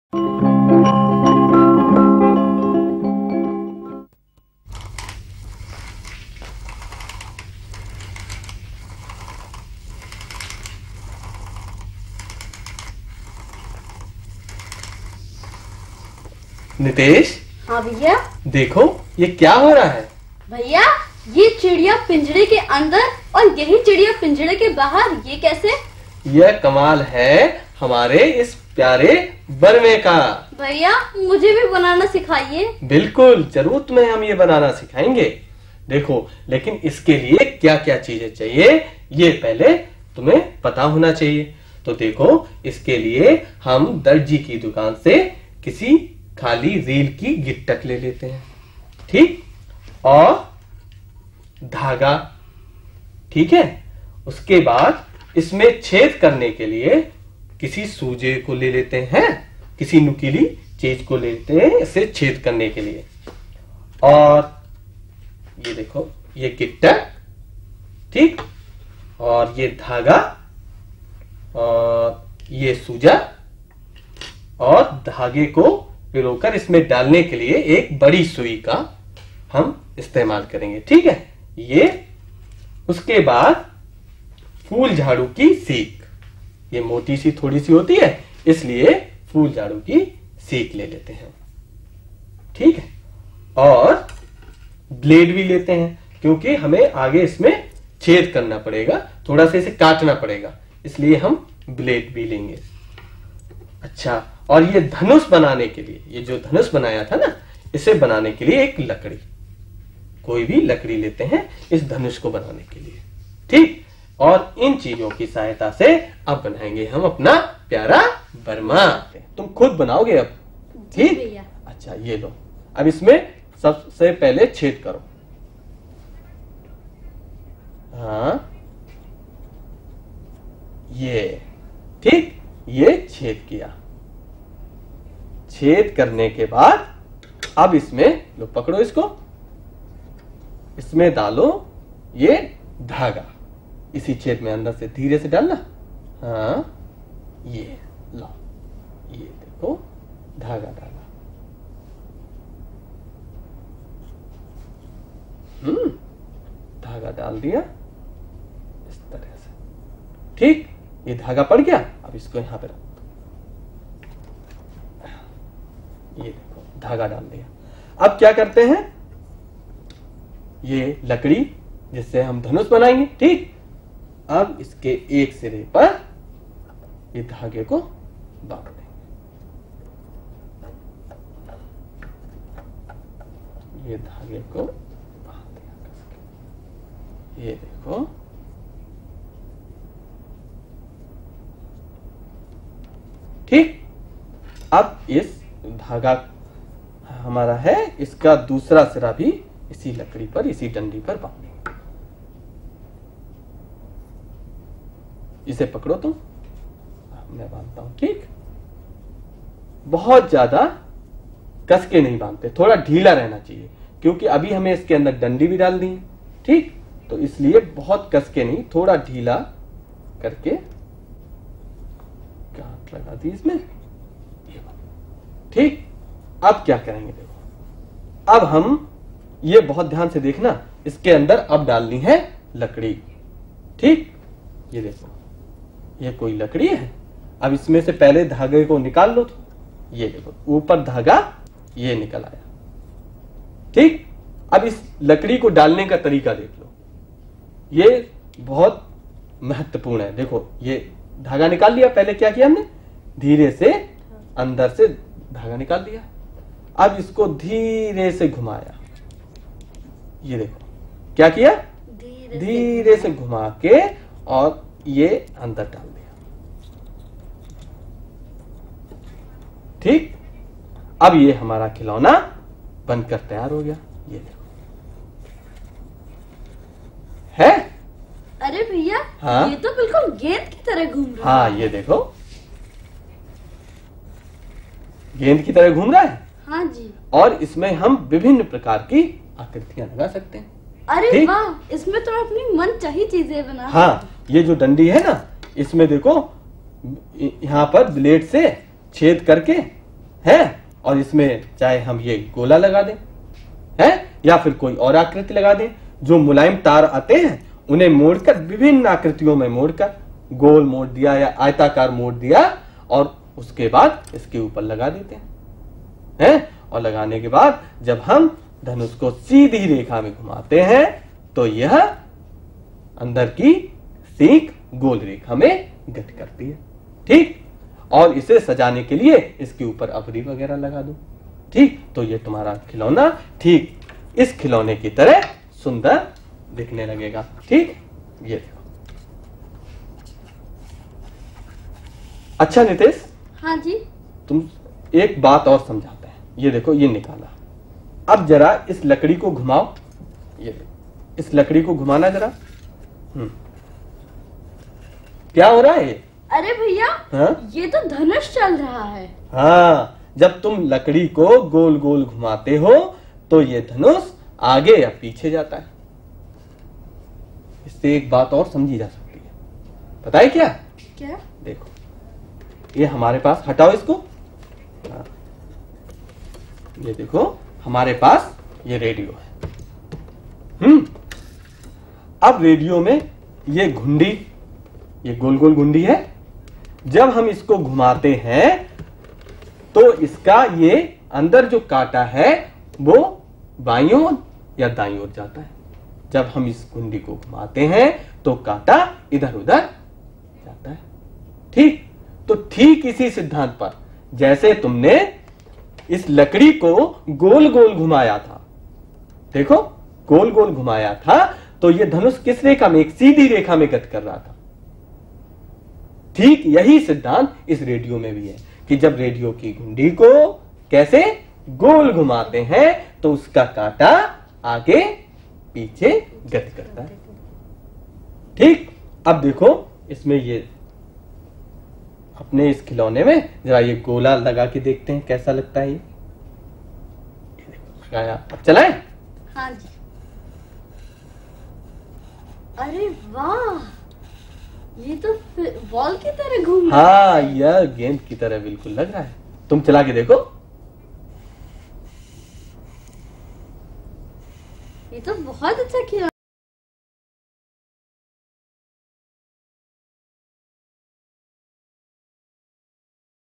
Pyara Barma / प्यारा बरमा
इस कार्यक्रम में बरमा खिलौना बनाने की क्रियाविधि को बताया गया है।